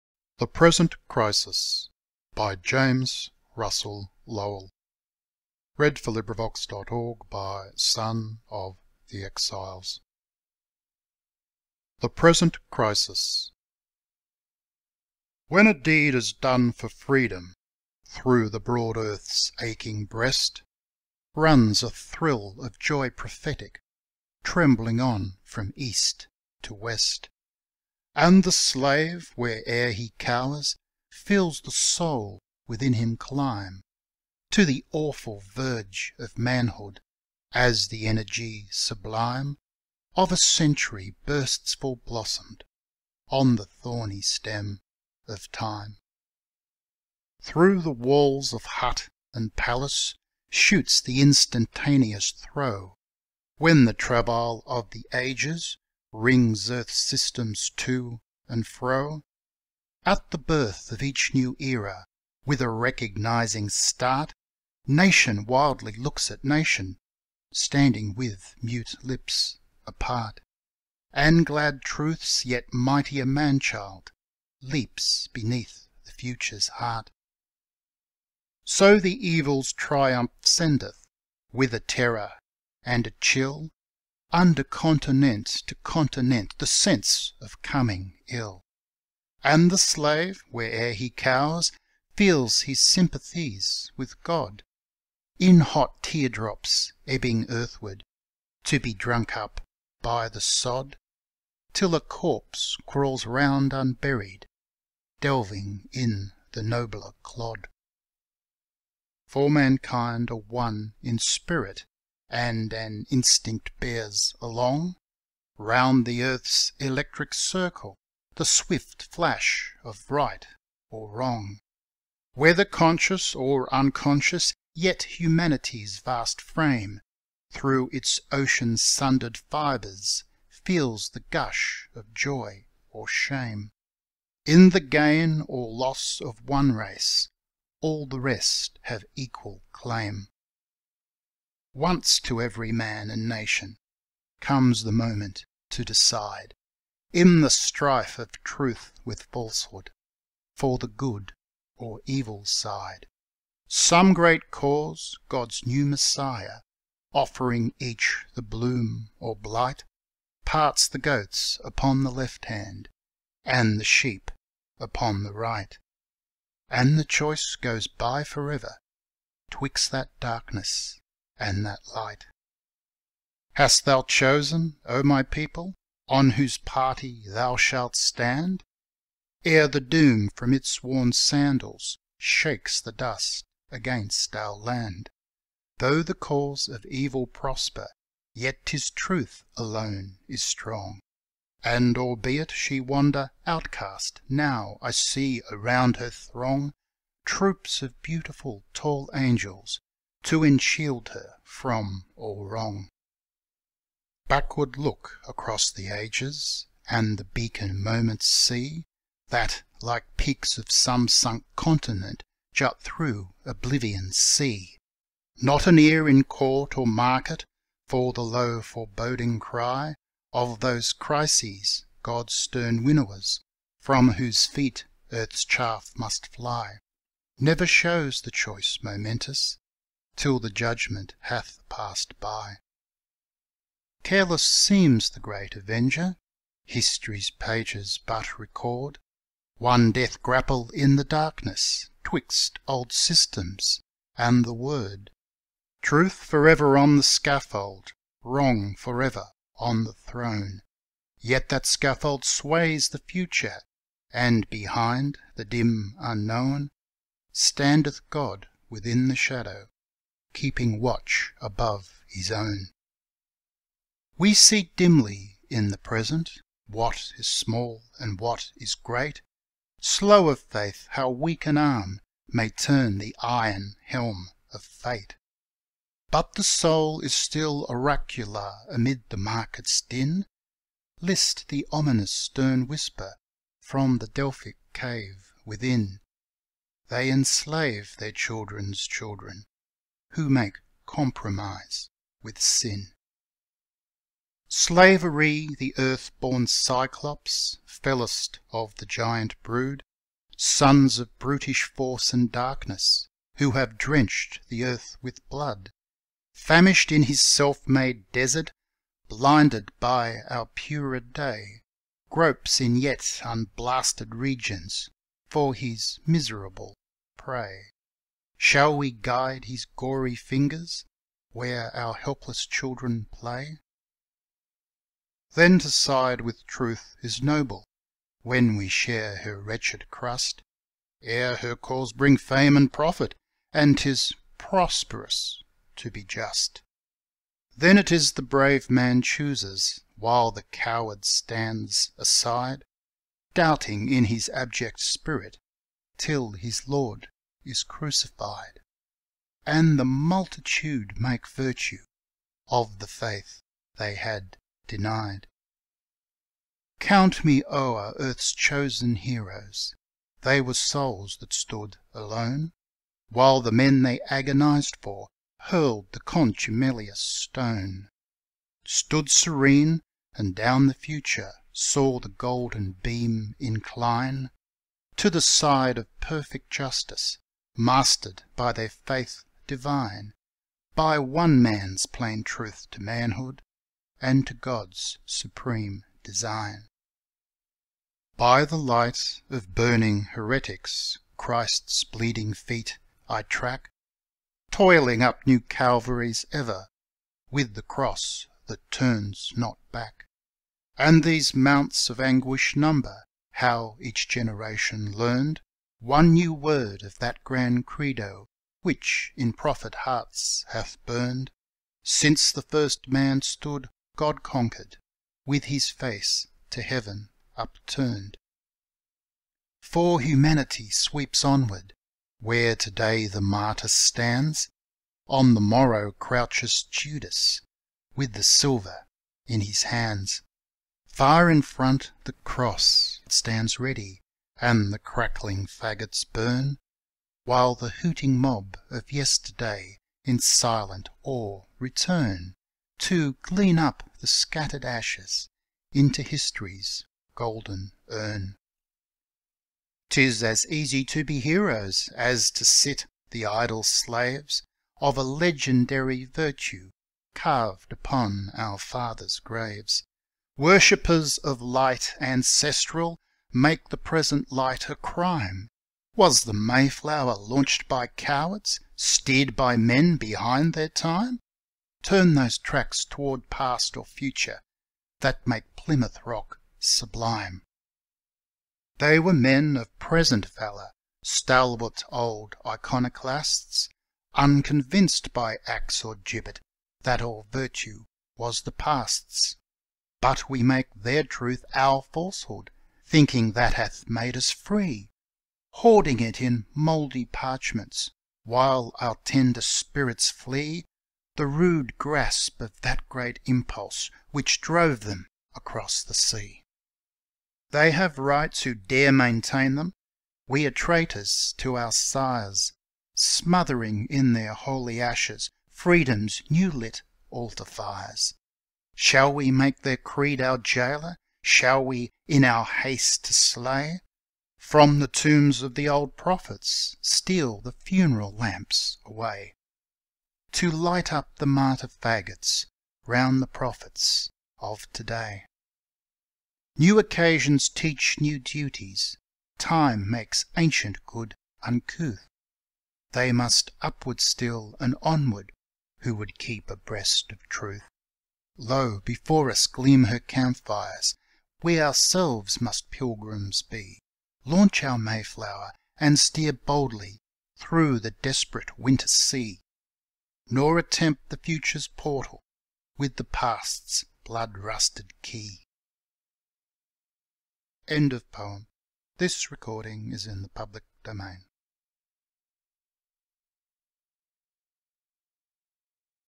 The Present Crisis by James Russell Lowell 1845
James Russell Lowell wrote this poem against slavery in 1845.